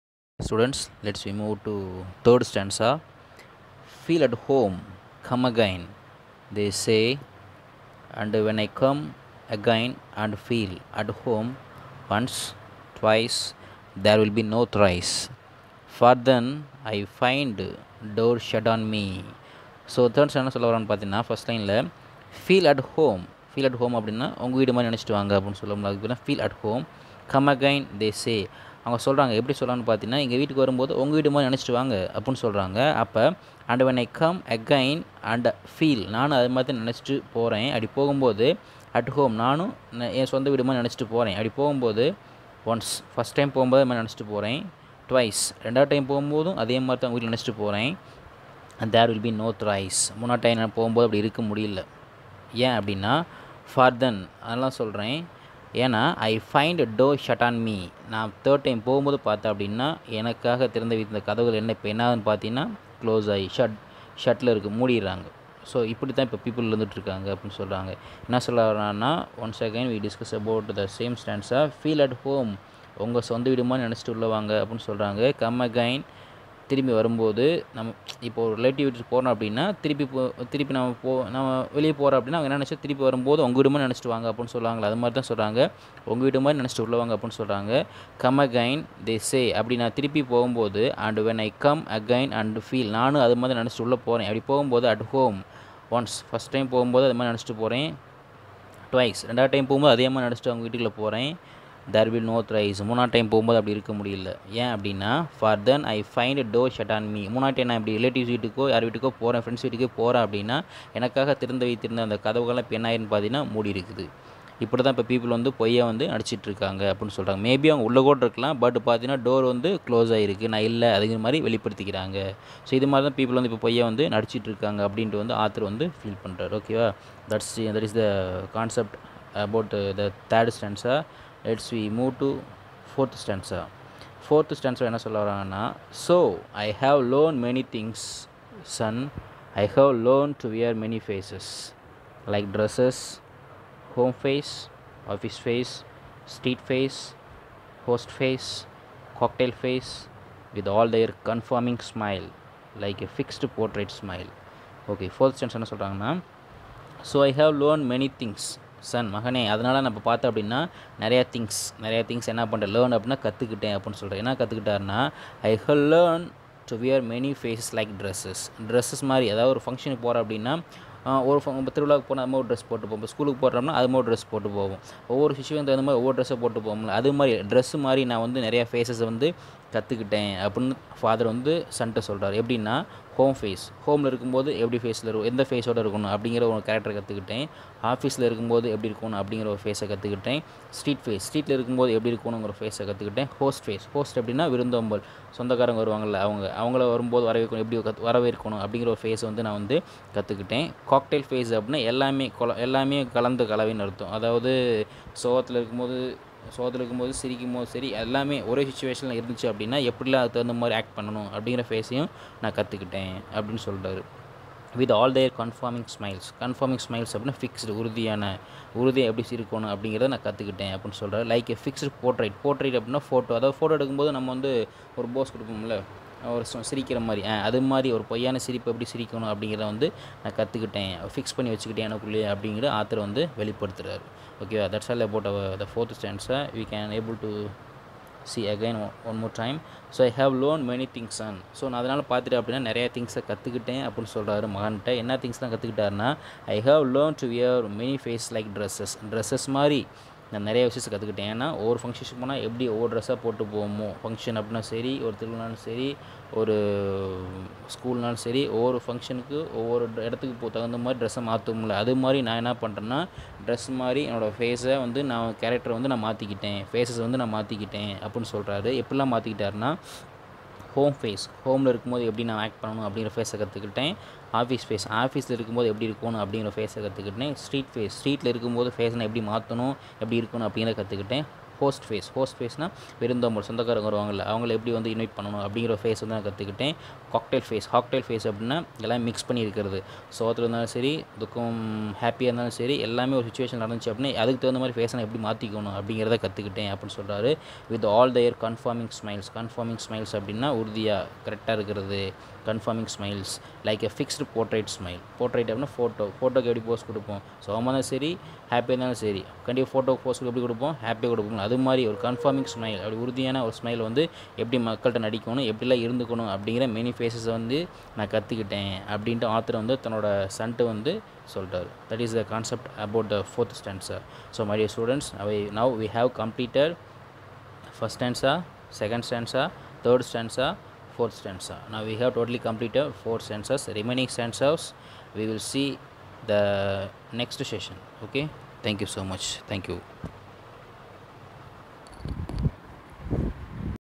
Once Upon a Time Author Name : Gabriel Okara (Part 3)Prepared by S.Manikandan M.A.,B.ed
Teaching of Poem (part 3)